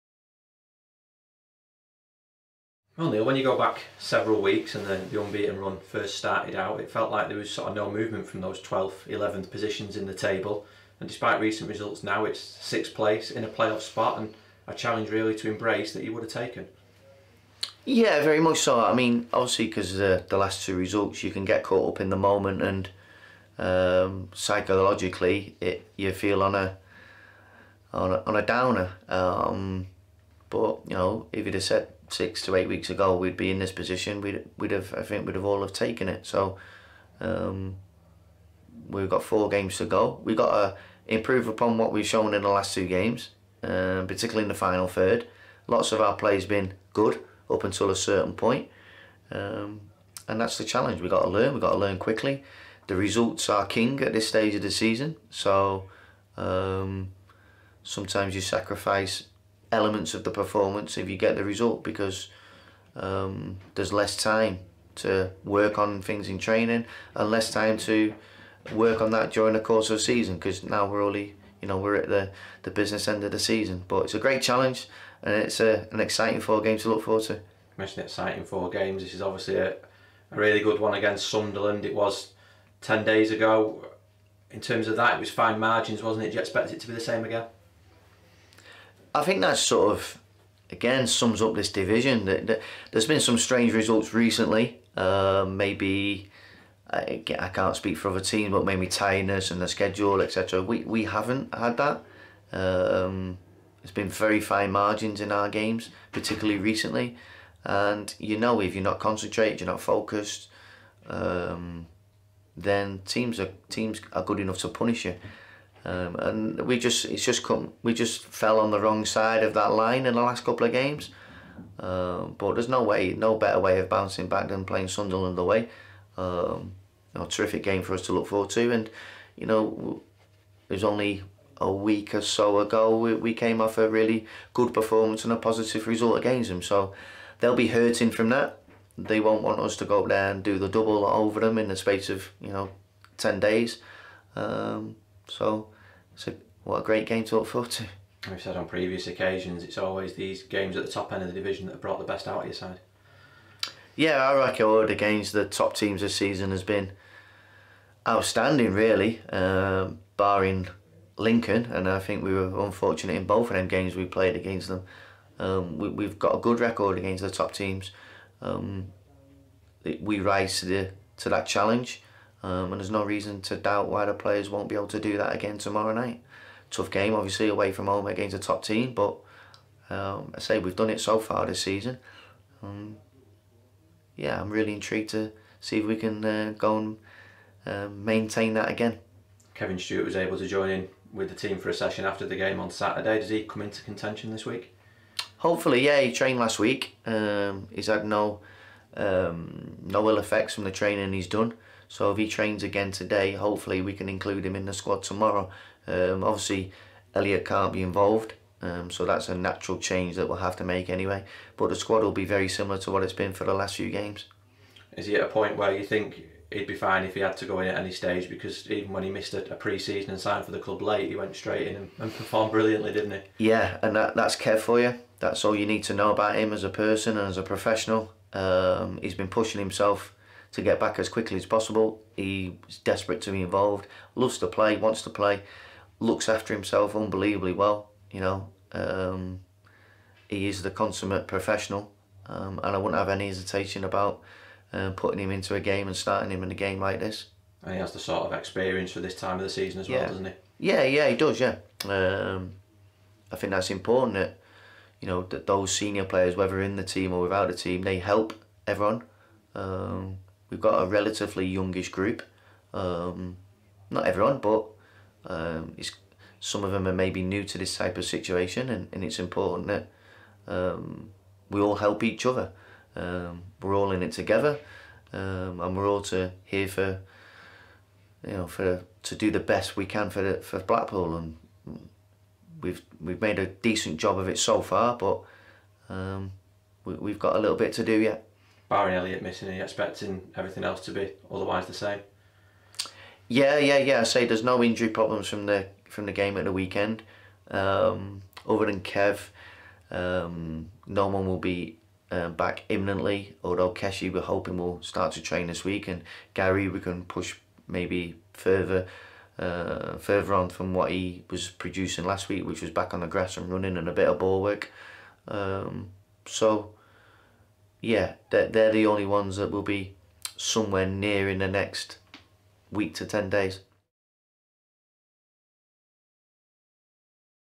"A great challenge and an exciting four games." | Neil Critchley | Sunderland Preview
Neil Critchley discusses Blackpool final run-in, starting with Tuesday night's trip to Sunderland.